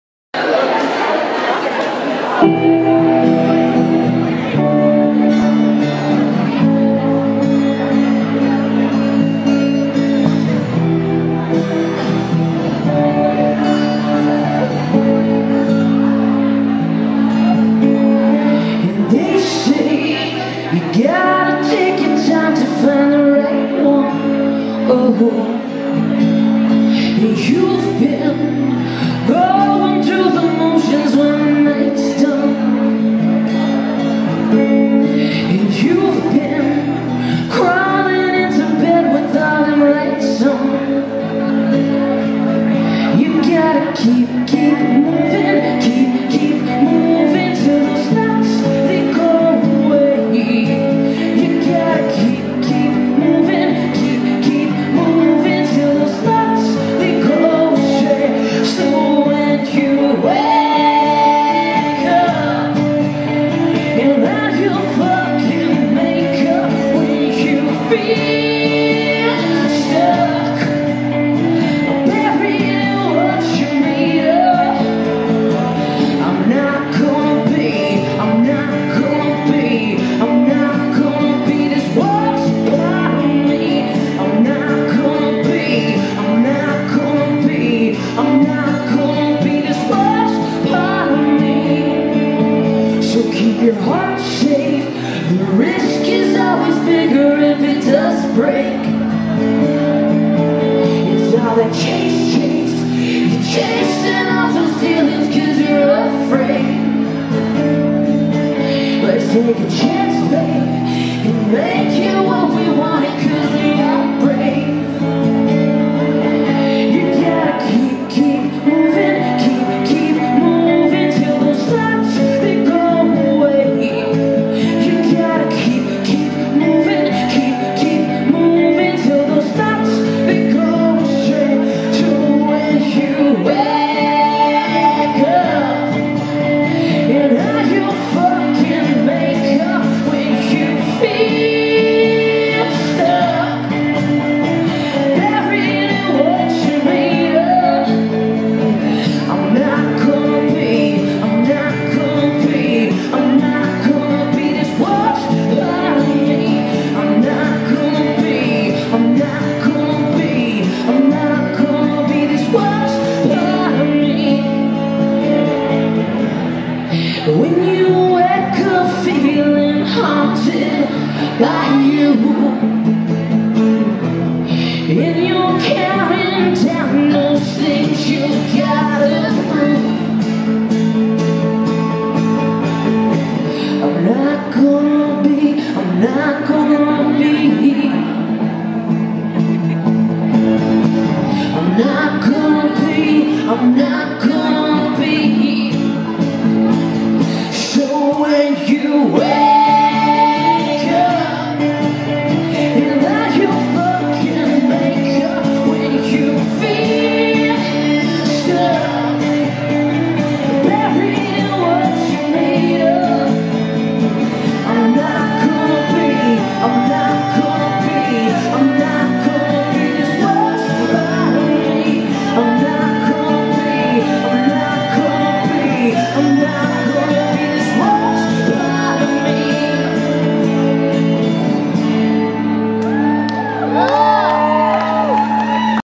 2/2 Kiyomi Valentine @ Phazefest, 9:30 Club, Washington, DC 9/04/15
Lead singer of Hunter Valentine, L Word season 3 cast member and LGBTQ super rainbow fighter extraordinaire